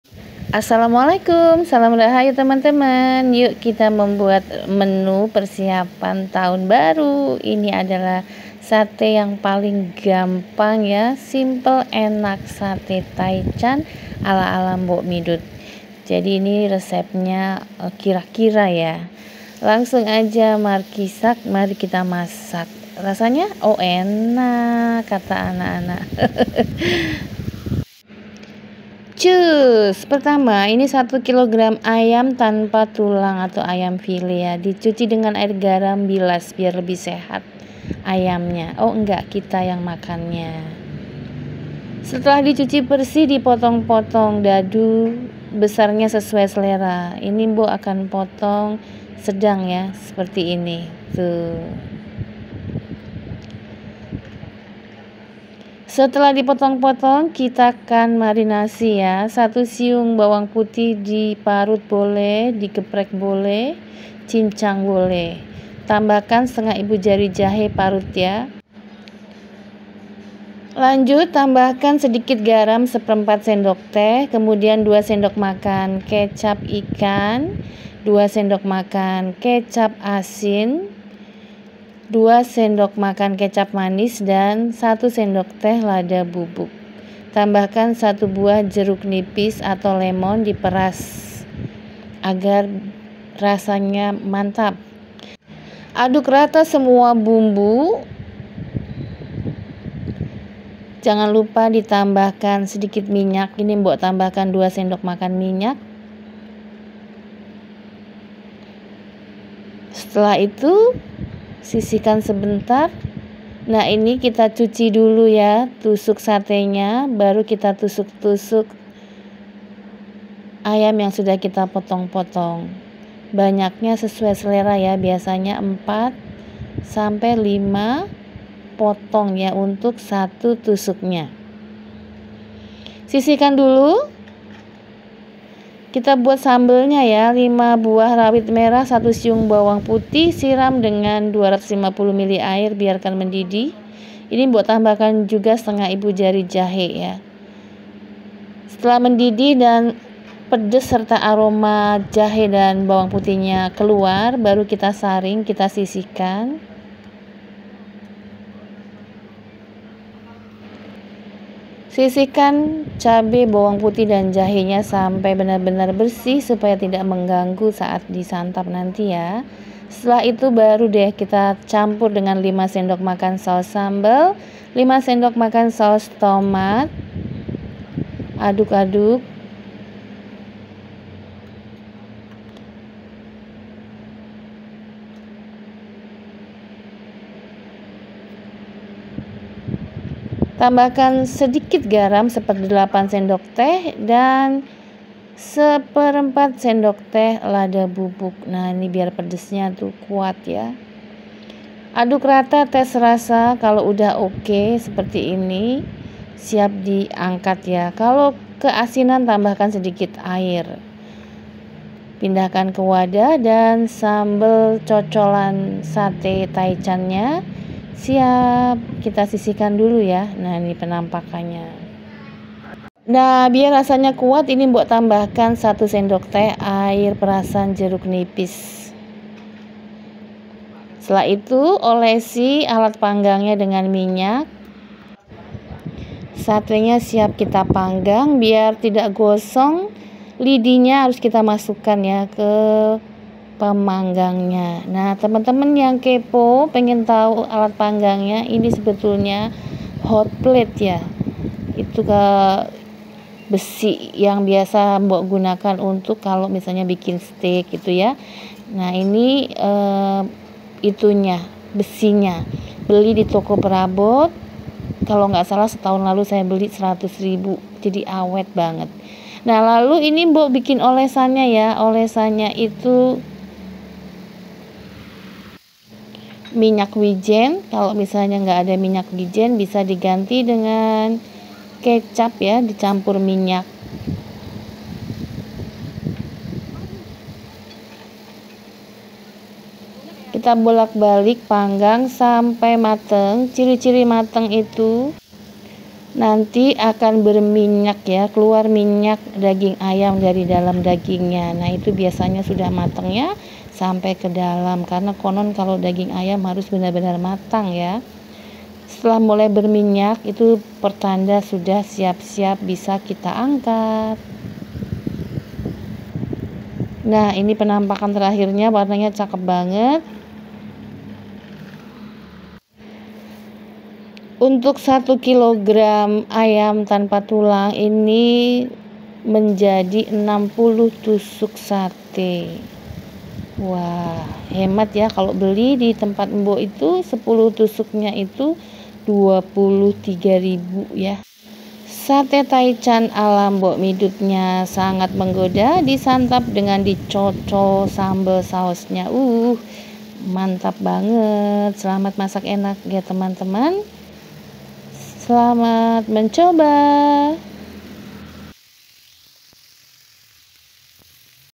DISAVE AJA DULU... EKSEKUSI TAHUN BARU
1 kg ayam tanpa tulang
60 tusuk lidi sate

1/2 ibu jari jahe
1 siung bawang putih
1 bh jeruk nipis/lemon
1/4 sdt garam
2 sdm kecap asin
2 sdm kecap ikan
2 sdm kecap manis
2 sdm minyak
1 sdt lada bubuk

5 bh cabai rawit
1 siung bawang putih
1/2 ibu jari jahe
300 ml air
1/8 sdt garam
5 sdm saos sambel
5 sdm saos tomat
1 sdt sari jeruk nipis/lemon

selamat mencoba🥰